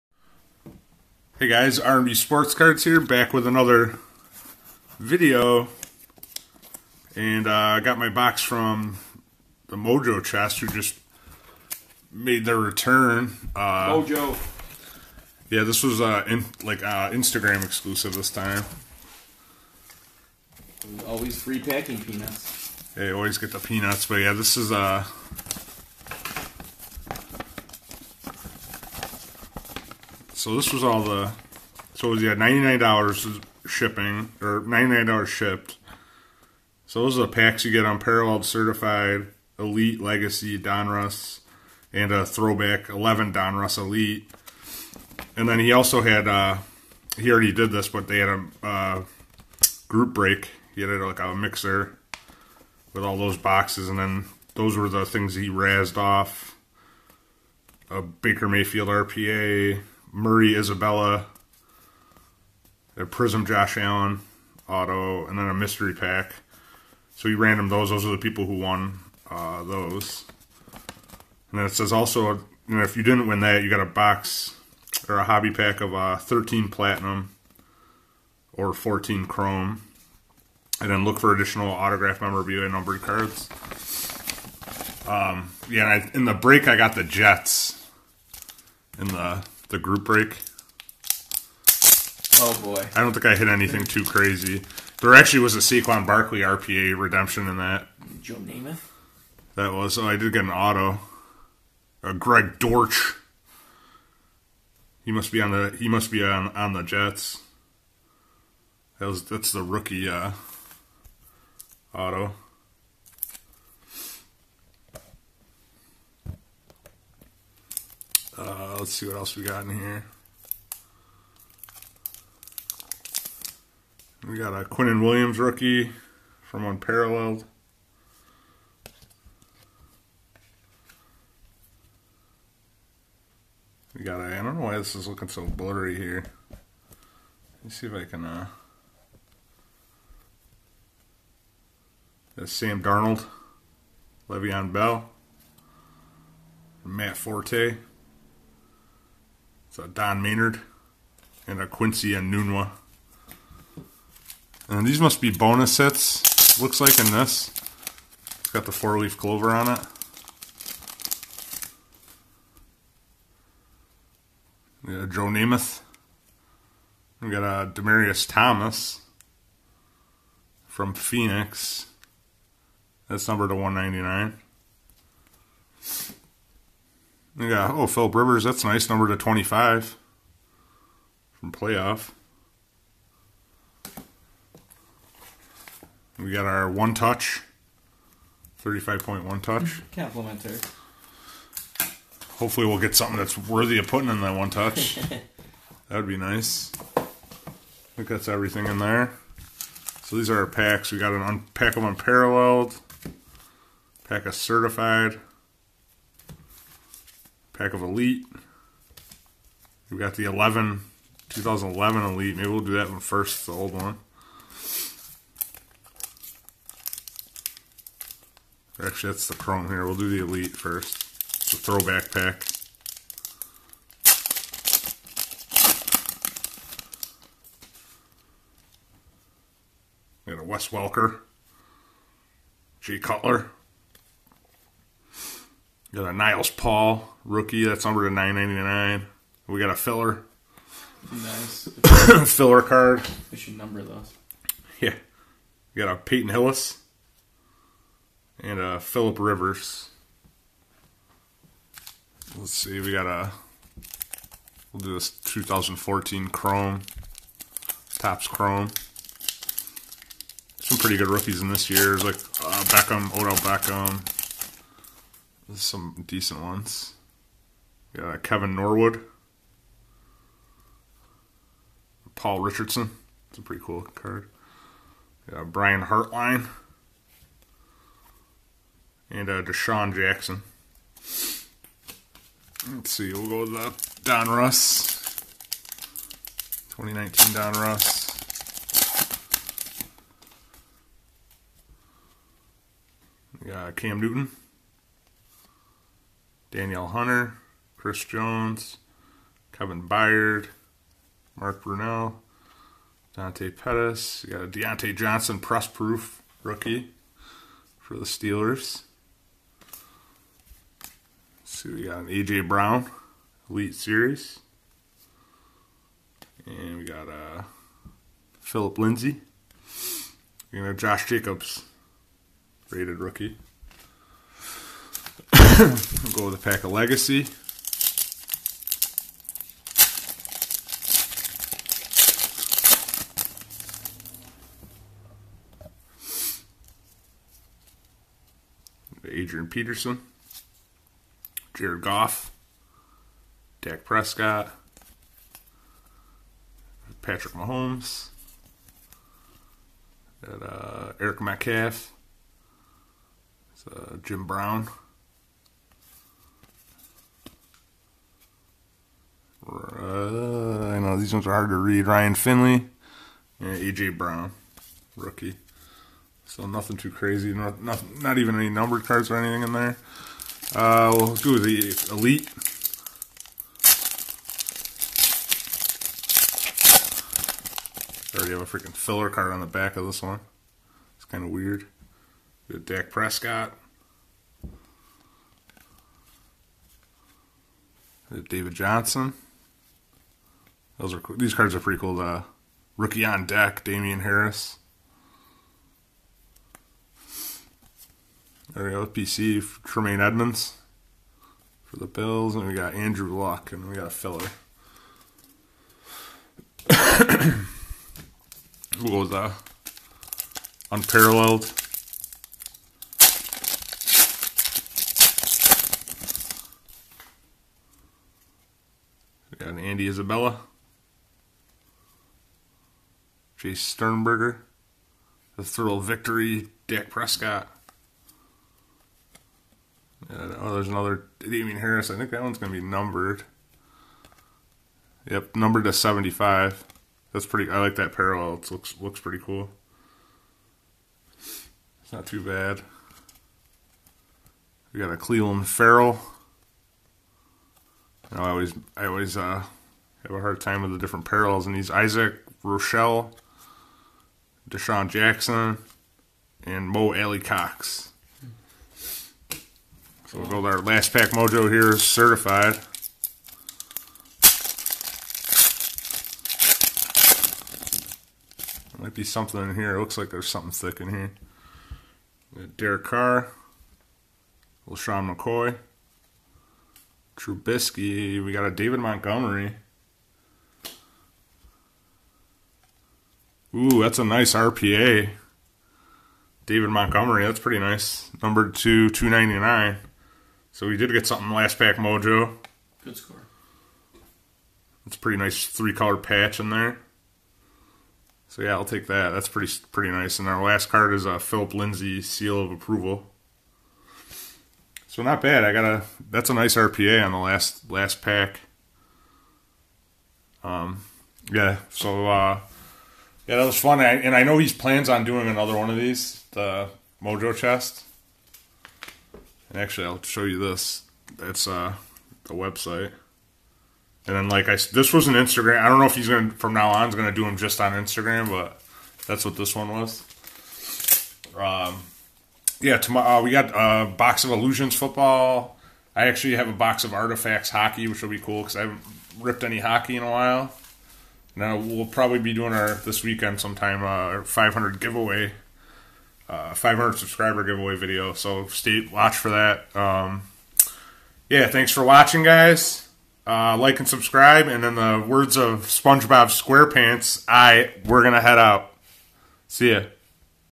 Return of the MOJO Chest!! Last Pack MOJO!!
The return of the MOJO Chest!! Nice Packs Sweet RPA Hit!!